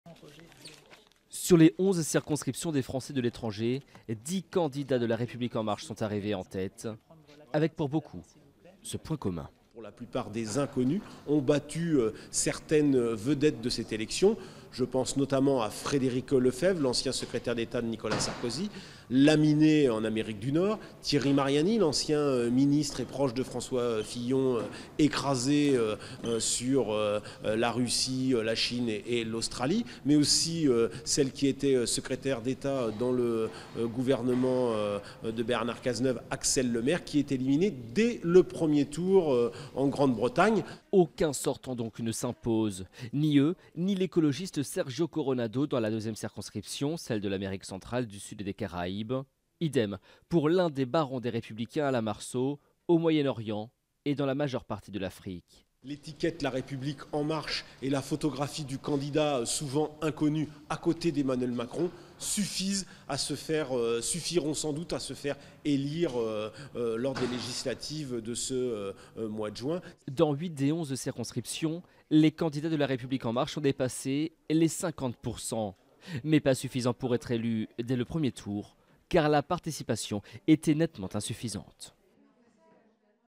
Raz-de-marée pour En Marche chez les Français de l'étranger
Les résultats du premier tour des élections législatives dans les circonscriptions des Français de l'étranger ont été dévoilés, ce lundi par les ministères de l'Intérieur et des Affaires étrangères. Dans 10 des 11 circonscriptions concernées, c'est le candidat de La République en marche qui est arrivé en tête. Dans 8 d'entre elles, le candidat soutenu par la majorité présidentielle a même obtenu plus de 50% des voix mais, faute de participation, un second tour sera partout nécessaire. En très mauvaises postures, des figures de LR, comme Frédéric Lefebvre et Thierry Mariani, ou du PS, comme Axelle Lemaire, ne devraient donc pas retrouver leur siège à l'Assemblée nationale.